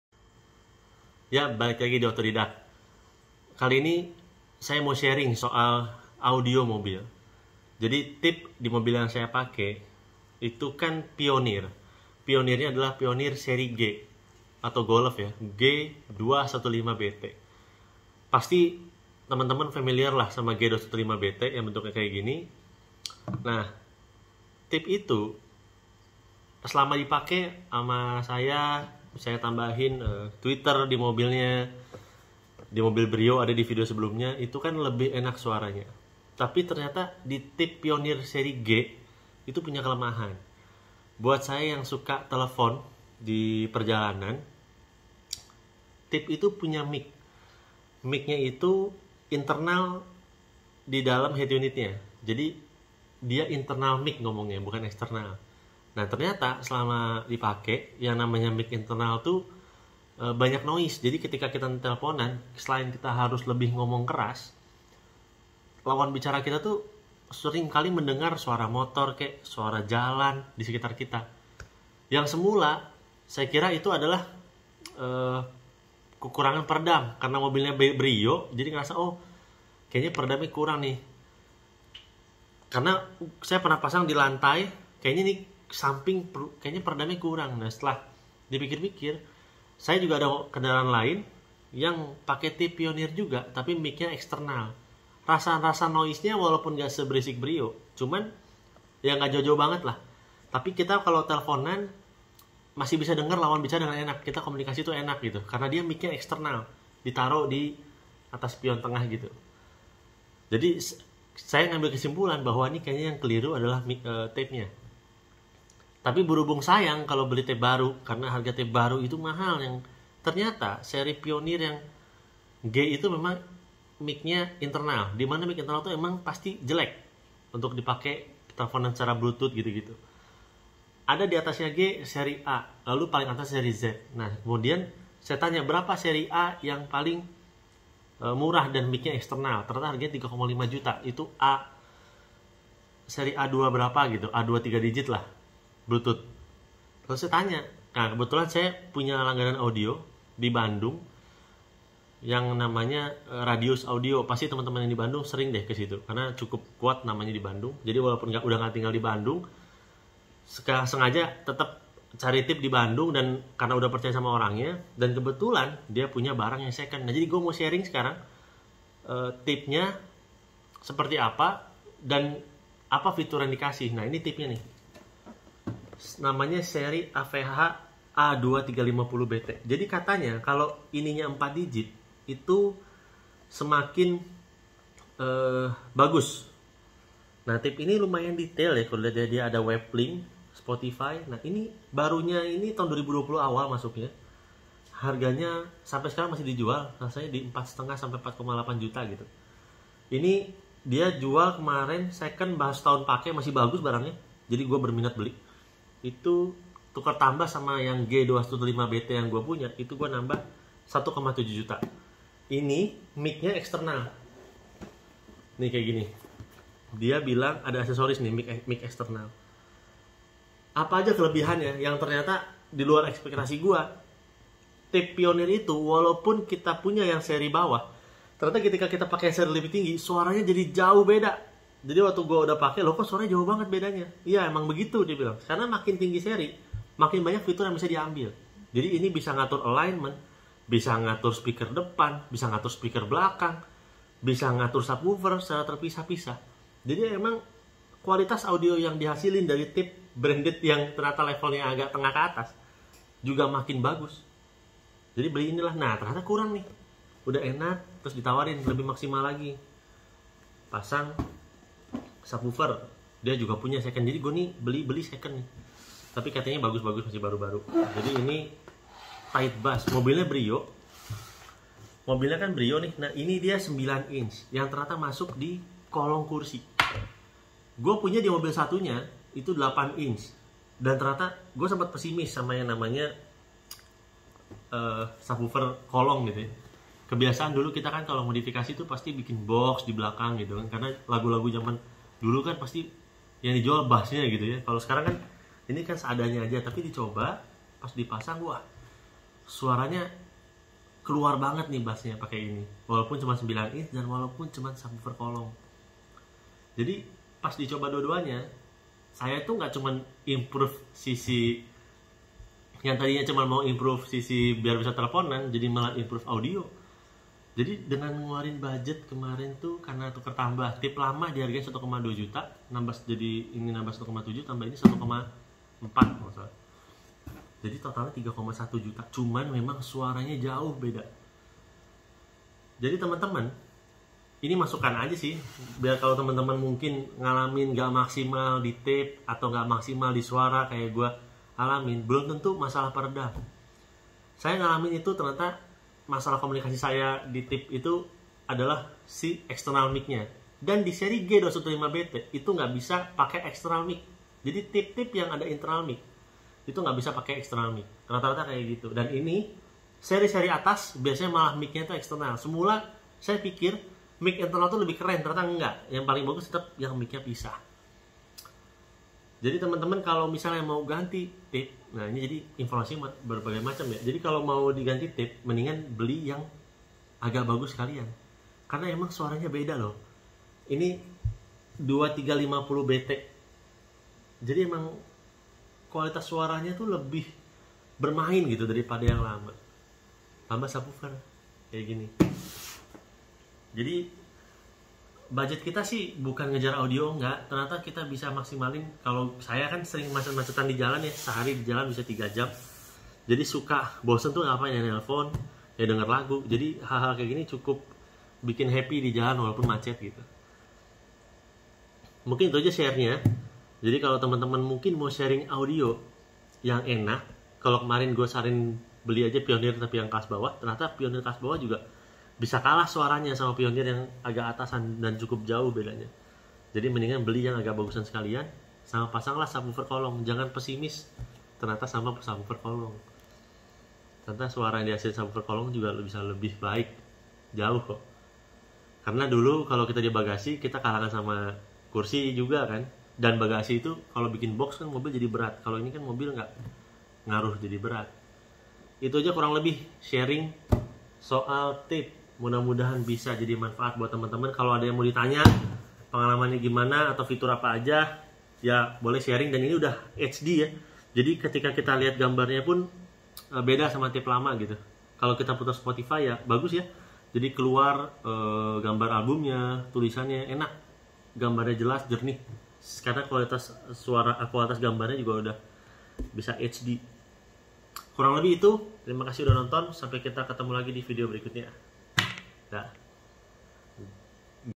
Berbagi Tips Fitur Mic dan Kualitas Suara Head Unit Pioneer A2350BT dan Subwoofer Tight Bass
Kali ini saya mau berbagi pengalaman menggunakan Pioneer low end yang ternyata kualitas suaranya menurun. Akhirnya di upgrade dari Pionner G215BT ke Pionner A2350BT.

selain itu buat teman2 yang suka menggunakan fitur telephony di headunit, di G215BT tdk akan maksimal karena mic nya merupakan mic internal. harus menggunakan HeadUnit yang menggunakan mic External. Seperti di tipe A2350 BT ini.

Selain itu saya juga menambah Subwoofer TightBass 9 inch yang suaranya cukup baik.

Semua saya dapat dengan barang second yang dibeli di Radius Audio Bandung.

Mohon maaf tdk bisa menampilkan perbandingan suara, jadi hanya berupa pendapat saja. Semoga tetep bermanfaat untuk temen2 semua dan silakan kalo mau diskusi soal rasa dan kualitas audio melalui kolom komentar.

Tetap sehat, tetap semangat!

salam